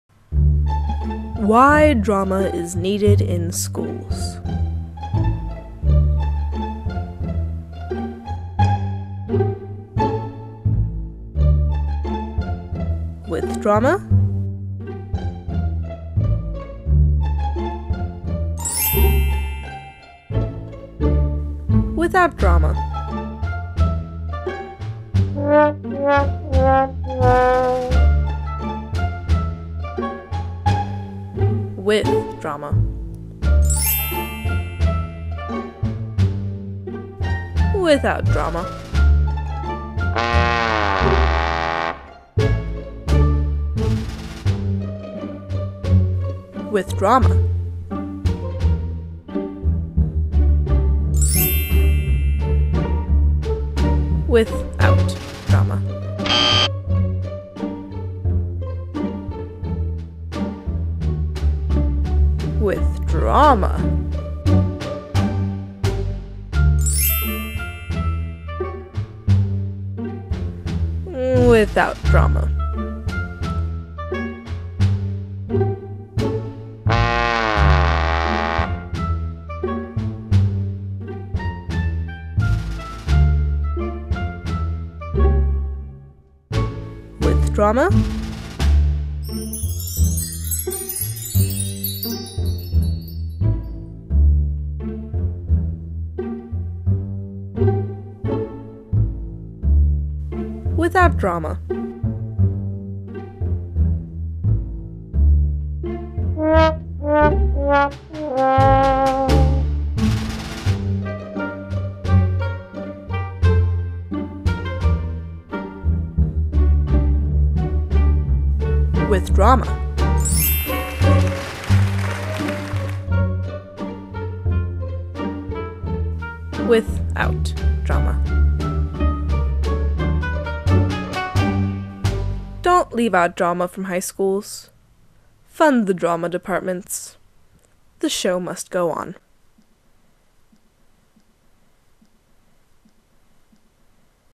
The Show Must Go On
Why drama is so important in school. Especially the drama class at New Technology High School. Features Raul Toscano as the star, Sierra Cootes as the Asian girl who is sometimes Raul's friend, Kyle Andrews as the guy who is sometimes Raul's friend, and Stephanie Sjoblom who bumps into Raul Toscano a lot and is generally mean to him. Filmed by Kyle Andrews. Edited by Stephanie Sjoblom.